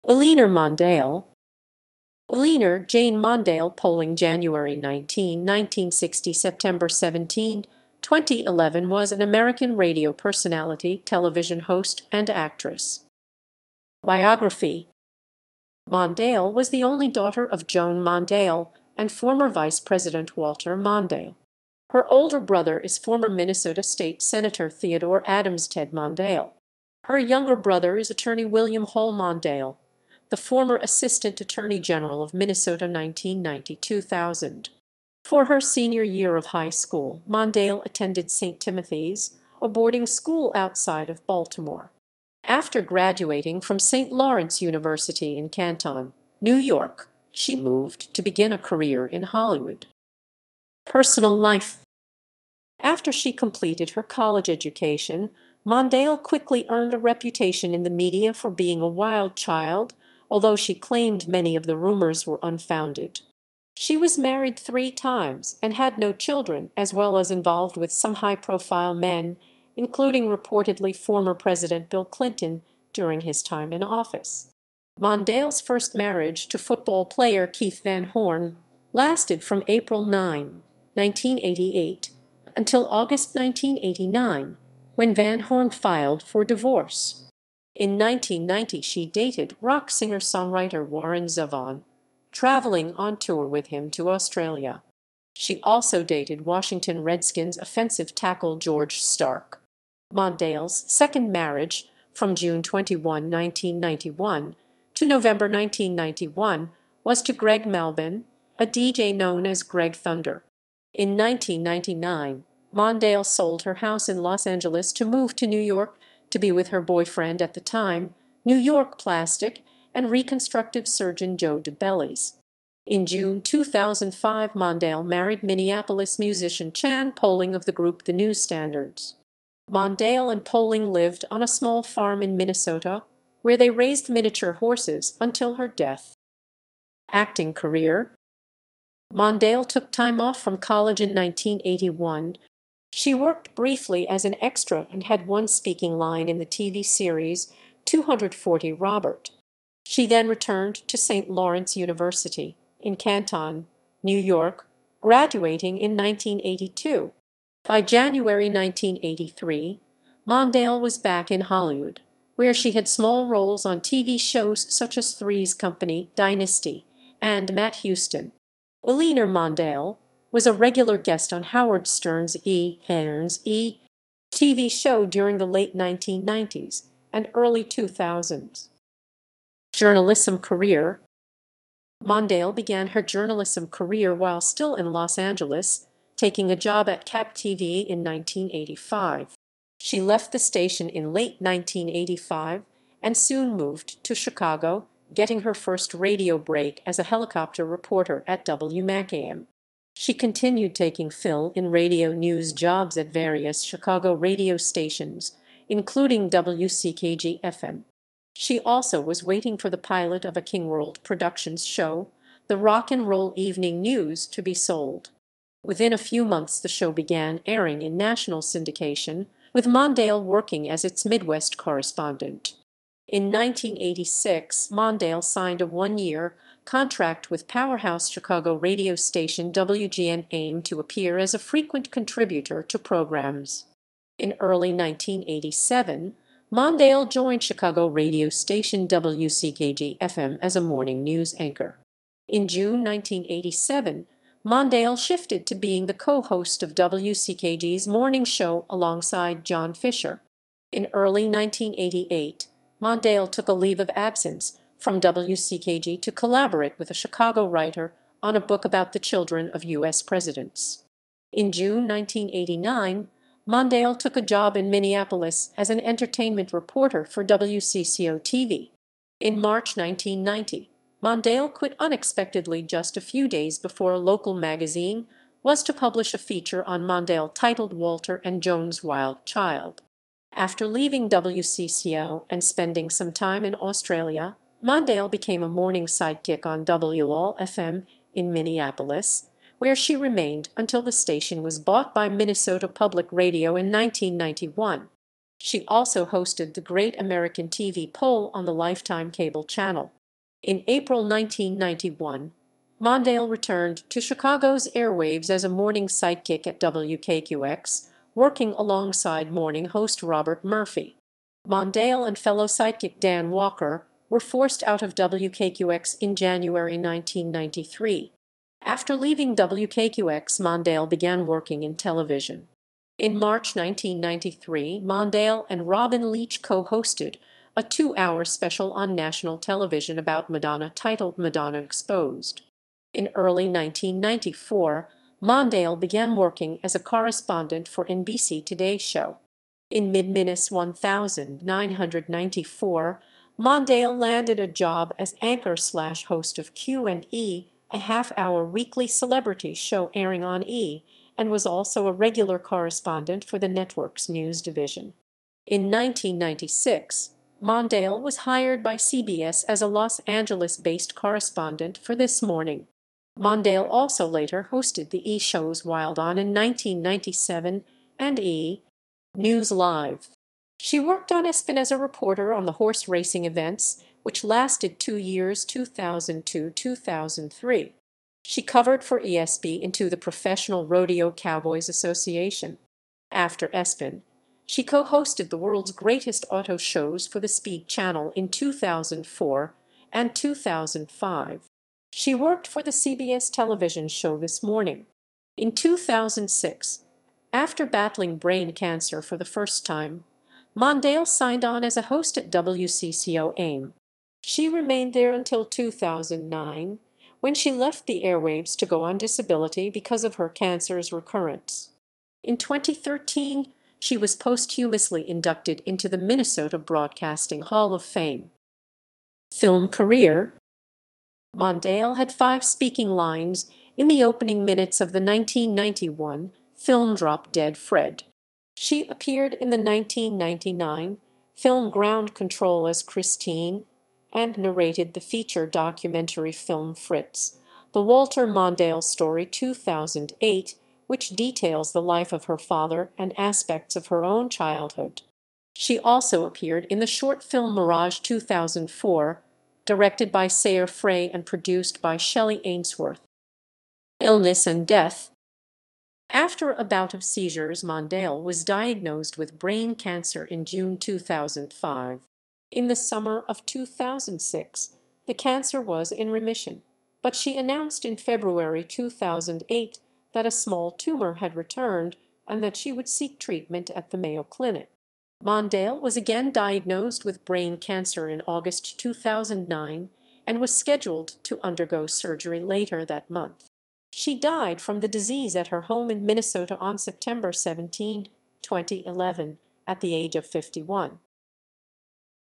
Eleanor Mondale
Eleanor Jane Mondale Poling (January 19, 1960 – September 17, 2011) was an American radio personality, television host, and actress.